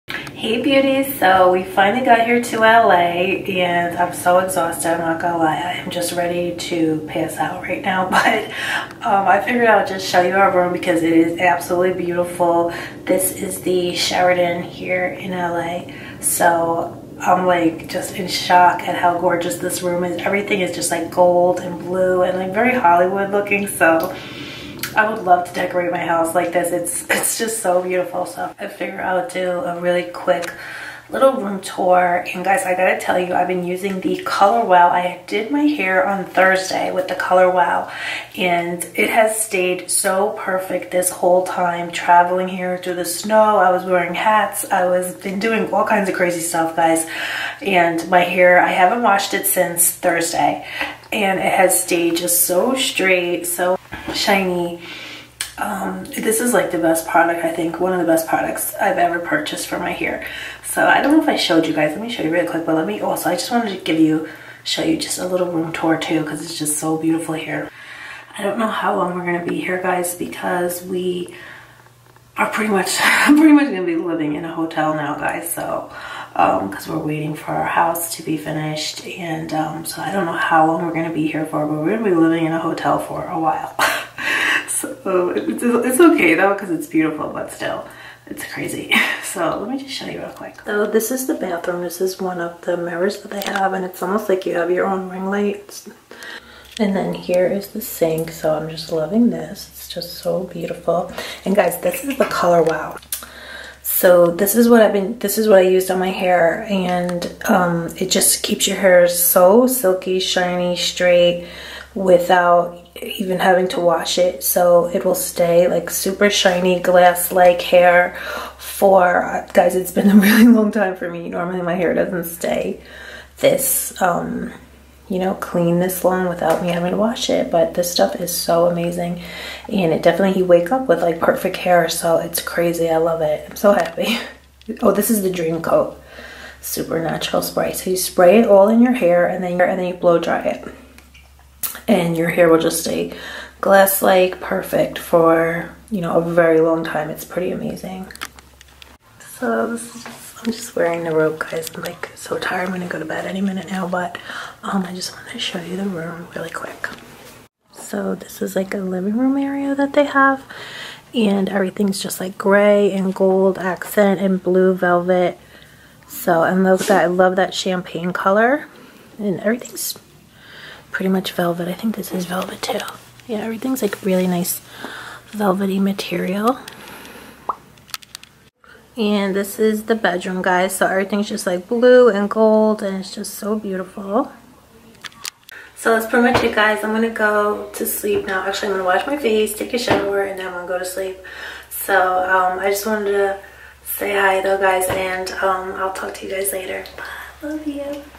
GLASS HAIR! SHINY STRAIGHT HAIR FOR DAYS at HOME TREATMENT!!!
for a new videos & to be entered in our monthly giveaways 💕👯 Hi beauties, I'm in LA finally and just wanted to share more on this amazing hair product Ive been loving! It's the Color Wow Dream Coat Spray, cannot believe how it has left my hair shiny, frizz free and glass like! Best of all, you can use it at home and lasts up to three washes.

COLOR WOW DREAM COAT SUPERNATURAL SPRAY SLAYS HUMIDITY AND PREVENTS FRIZZ

Love,
Alegra